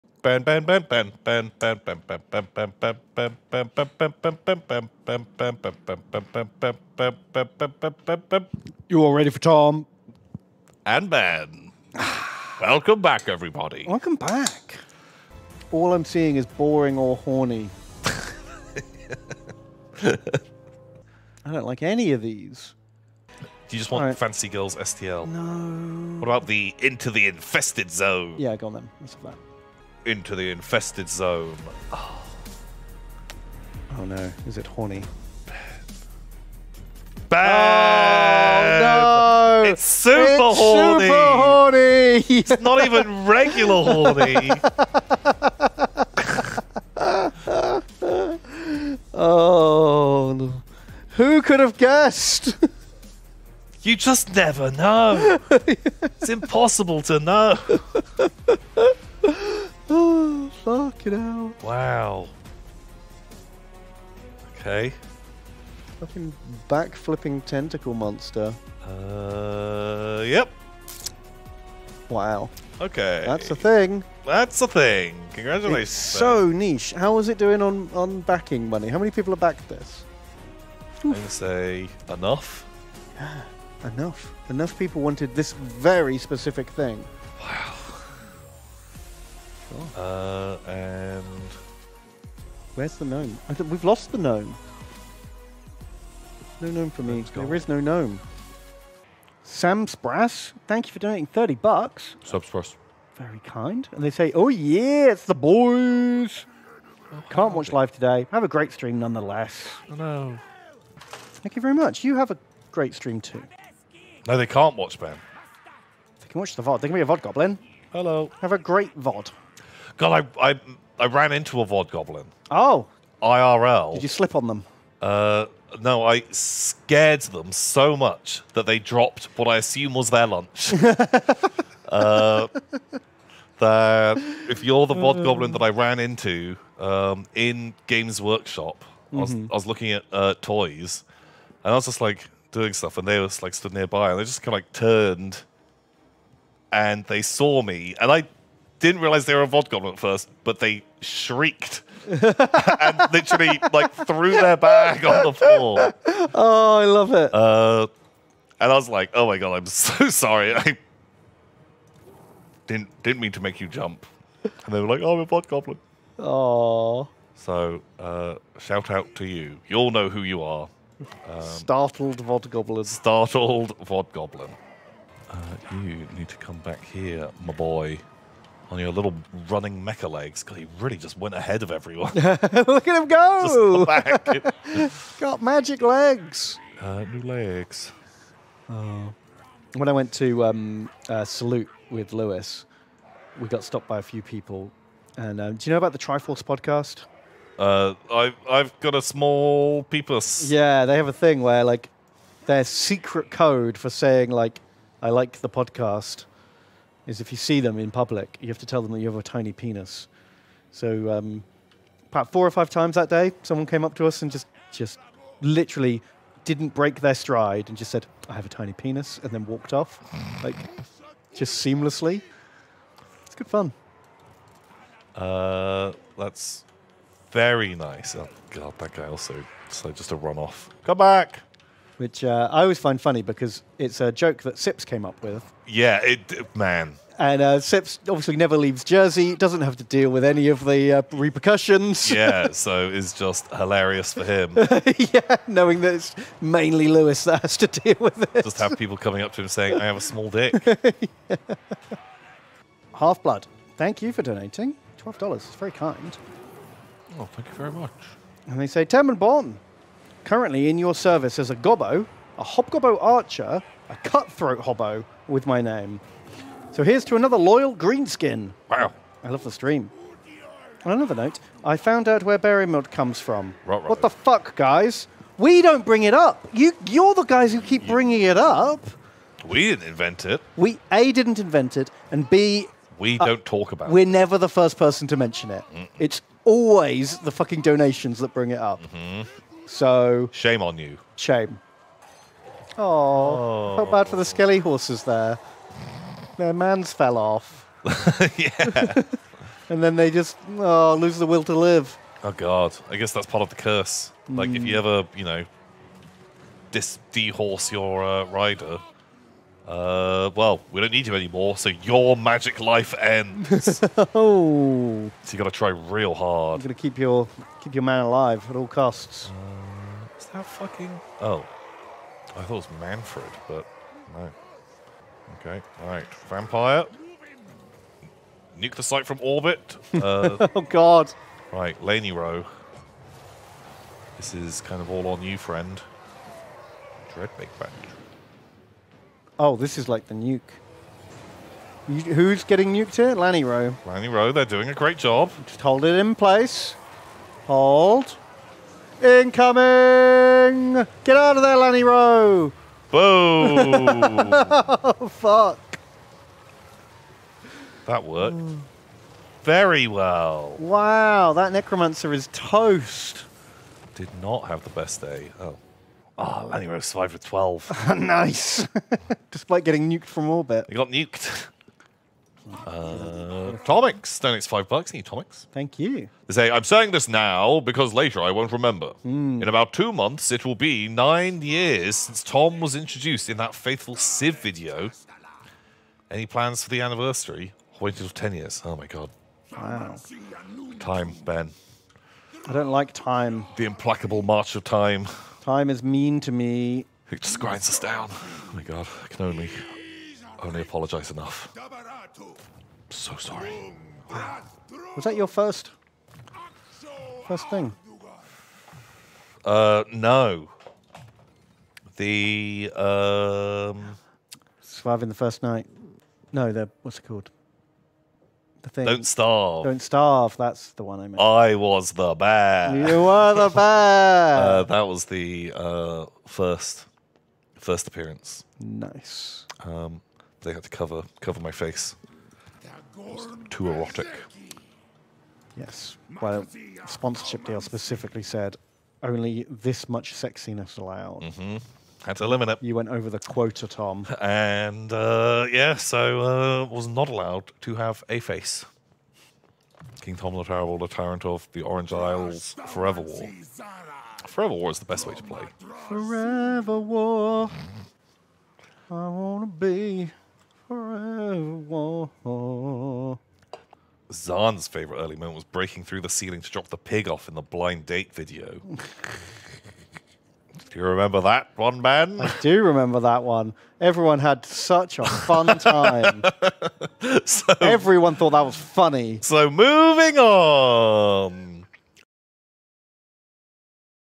Tom Scares A VOD Goblin - Ben Meets Triforce Fans - Tom's First Appearance on the Yogscast
Kickstarter Roulette is full of surprise h*orny.

00:00 Tom and Ben Intro
00:15 Into the Infested Zone
01:39 Tom Scares A VOD Goblin
02:55 Magic Mecha Legs
03:09 Ben Meets Triforce Fans
04:12 Berry Milk
04:52 Soulbound Skeleton Horses
05:20 Rain Fire
06:12 Tom's First Appearance on the Yogscast